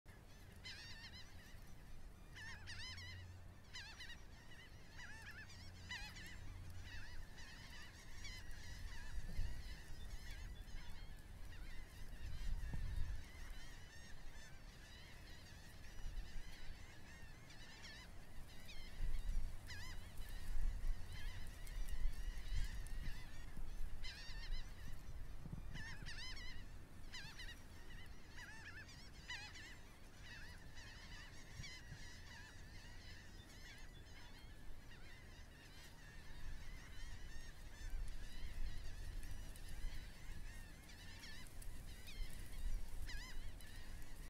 Fox Kit Pounce - Minute of Wildlife
Cute Red Fox kits play with each other on the lawn of a home in an urban environment in New Jersey.